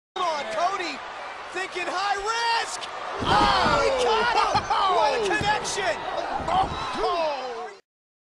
SLS: Cody Rhodes Moonsault Attack (Stop Motion Animation)
This video is my attempt at using the go motion feature in Dragon frame my new program let me know what you think about it in the comment section Below...

Published 3/13/14 ____ GO MOTION

SLS Wrestling Animation™